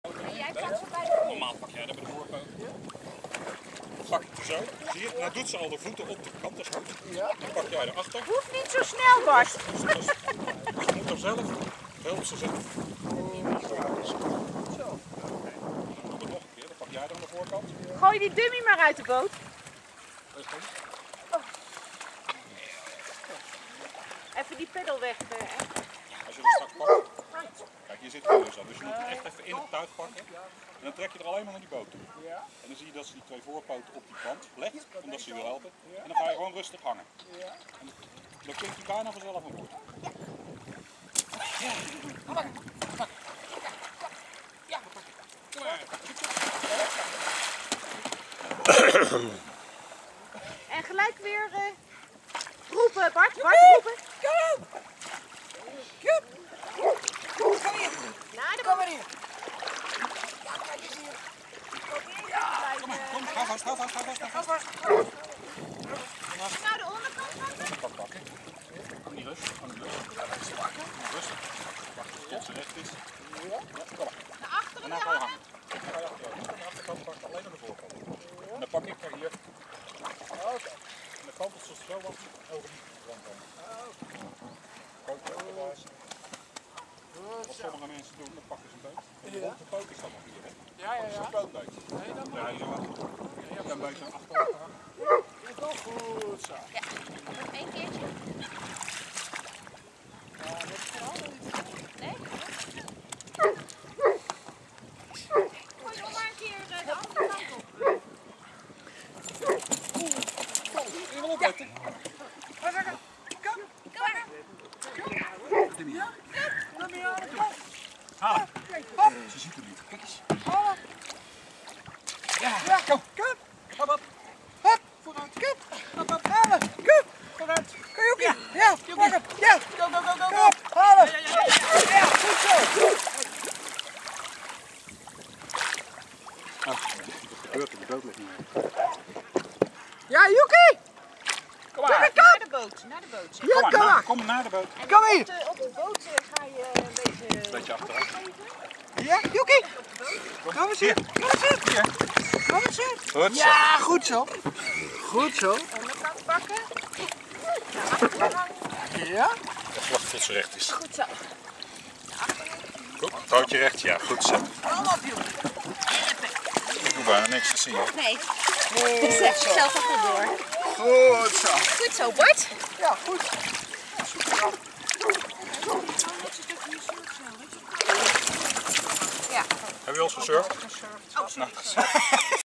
NF Kringgroep 's-Gravenhage 2014. Yuki
Baas moet leren hoe hij zijn hond op de best manier de boot in kan trekken.